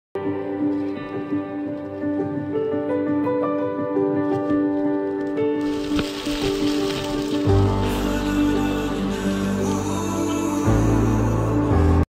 📍 Monarch Butterfly Biosphere Reserve, Mexico
A 56,259-ha biosphere reserve known as the Monarch Butterfly Biosphere Reserve is located about 100 km northwest of Mexico City in the rough, wooded highlands.
It was established to safeguard the Monarch Butterfly's wintering grounds since it is arguably the most well-known of all North American butterflies. ✨

Would you like to visit this reserve?